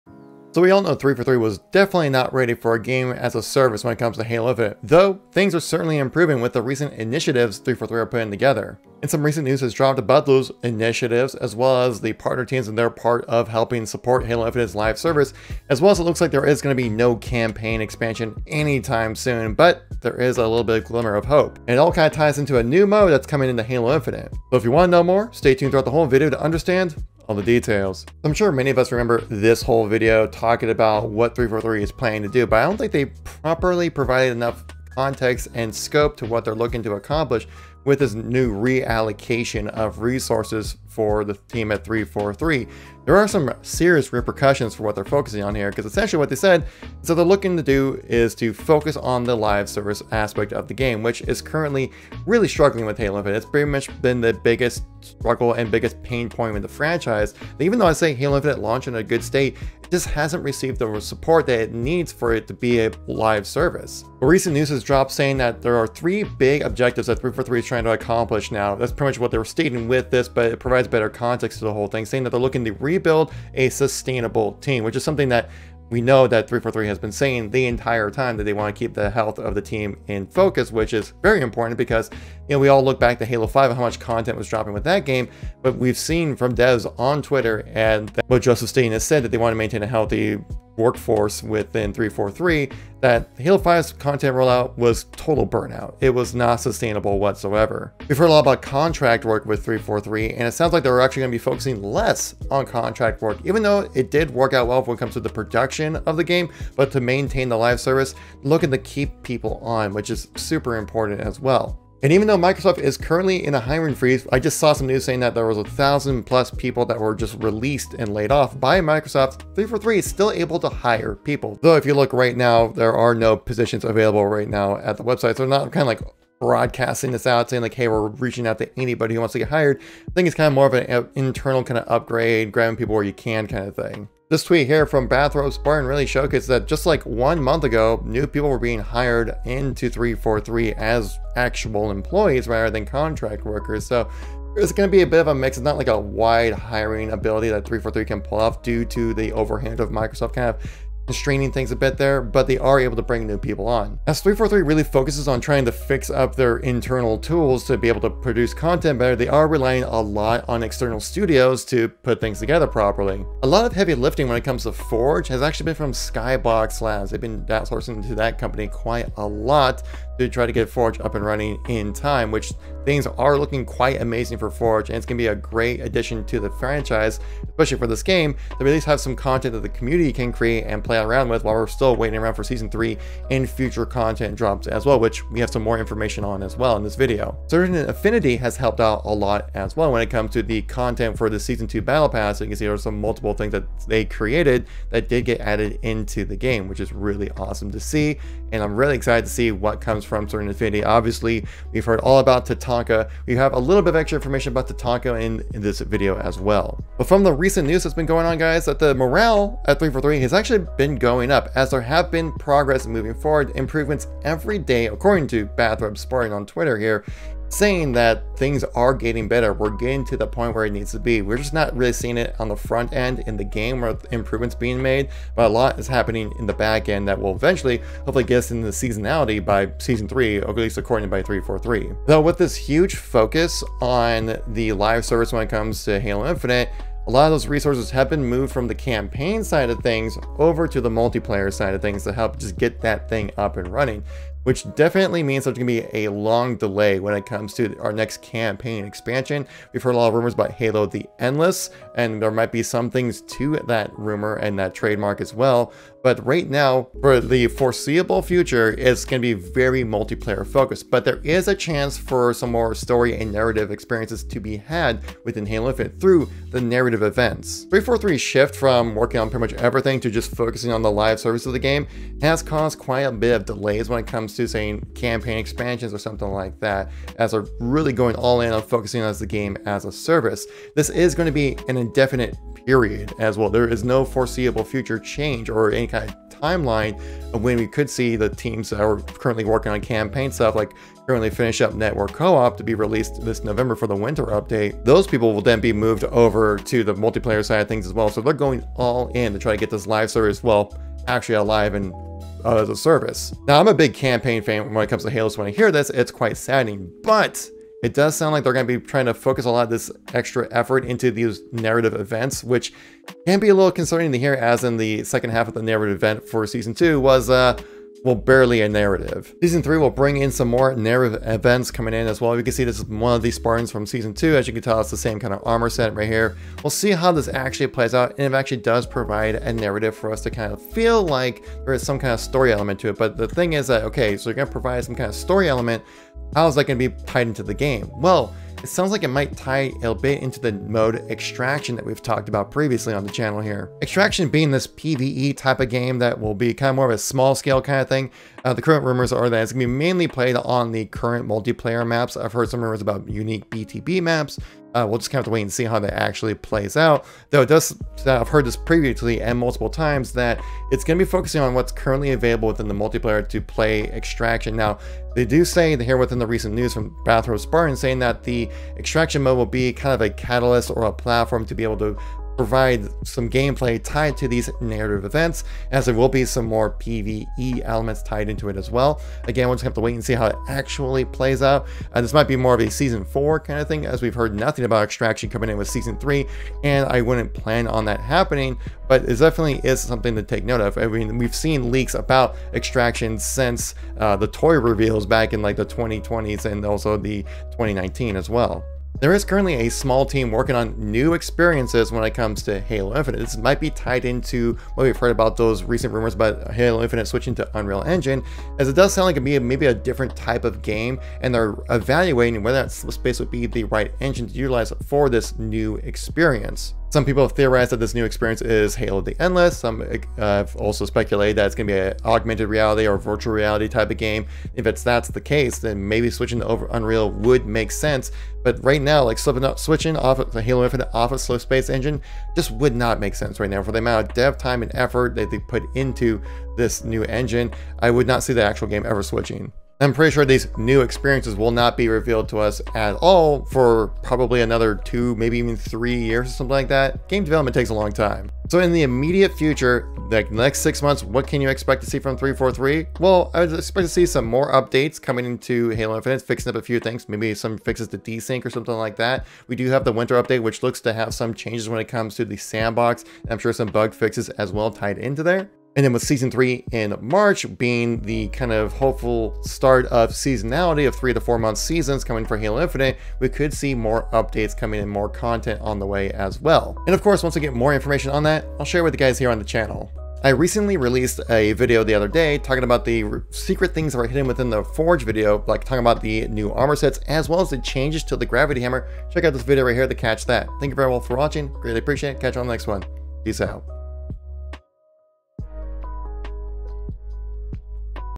343 Drops Halo Infinite Campaign DLC for Live Service Indefinitely! Halo News
343 Drops Halo Infinite Campaign DLC for Live Service Indefinitely!